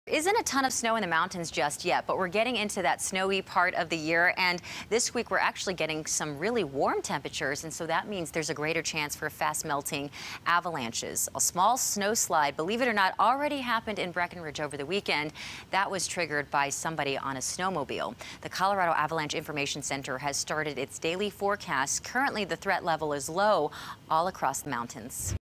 Snowmobiler Triggers Snow Slide Near Breckenridge, Avalanche Threat Level Currently Low In Colorado
There is a greater chance for fast melting snow and avalanches in Colorado's mountain areas this week.